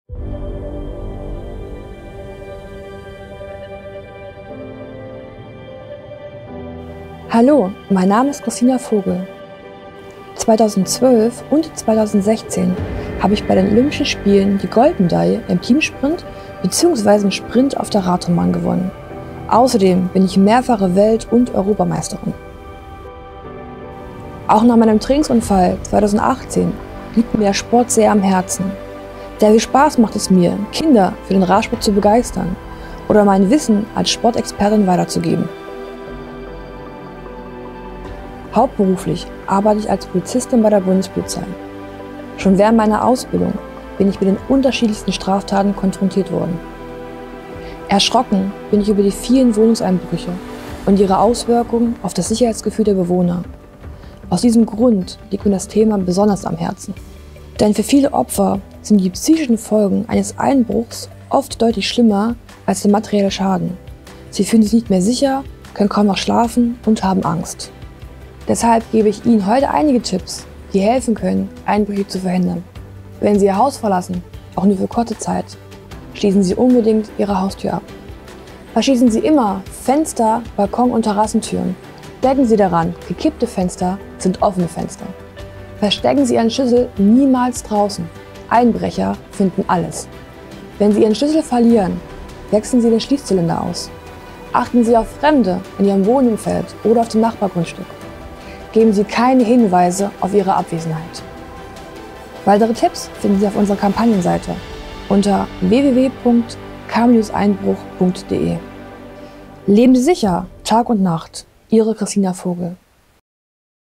Kristina Vogel - Botschafterin der Einbruchschutz-Kampagne K-EINBRUCH
Olympiasiegerin und Polizistin Kristina Vogel ist Botschafterin unserer Einbruchschutzkampagne K-EINBRUCH. In ihrem Botschafter-Spot gibt sie Verhaltenstipps zu effektivem Einbruchschutz.